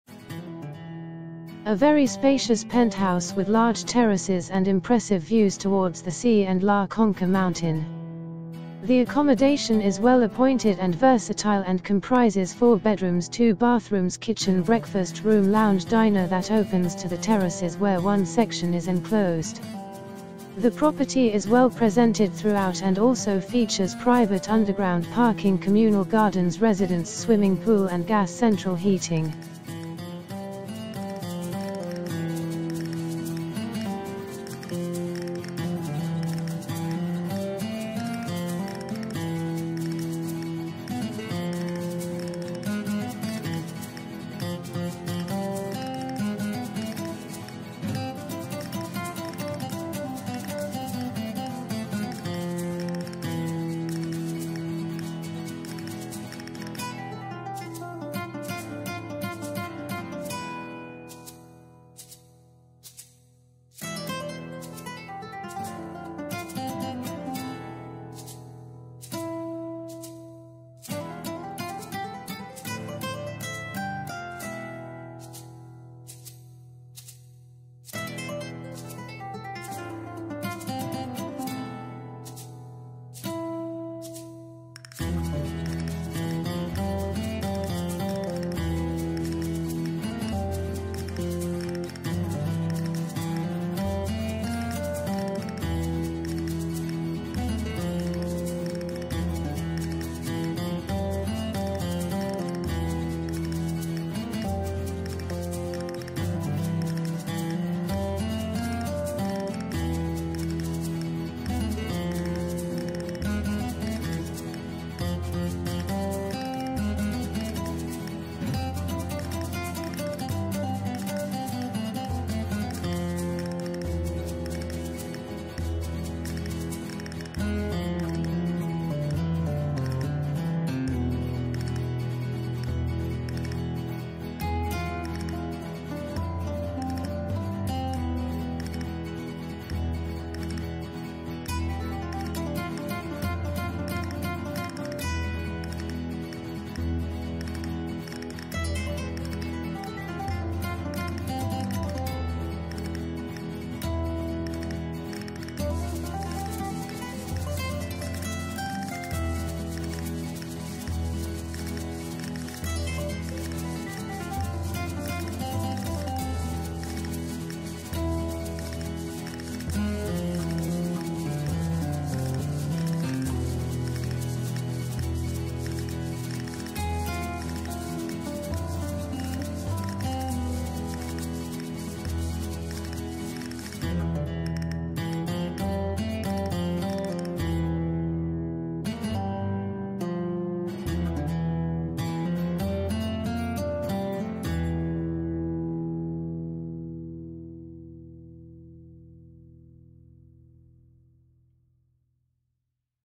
4 Bedrooms Middle Floor Apartment in La Campana, Costa Del Sol FOR SALE €370,000
4 Bedrooms Middle Floor Apartment in La Campana, FOR SALE €370,000 Property ID: CDS17669
A very spacious penthouse with large terraces and impressive views towards the sea and La Concha mountain. The accommodation is well appointed and versatile and comprises four bedrooms, 2 bathrooms, kitchen breakfast room, lounge/diner that opens to the terraces where one section is enclosed. The property is well presented throughout and also features private underground parking, comunal gardens, residents swimming pool and gas central heating.Fitted Wardrobes;Near Transport;Private Terrace;BarClose To Port;Close To Shops;Close To Sea;Close To Town;Close To Schools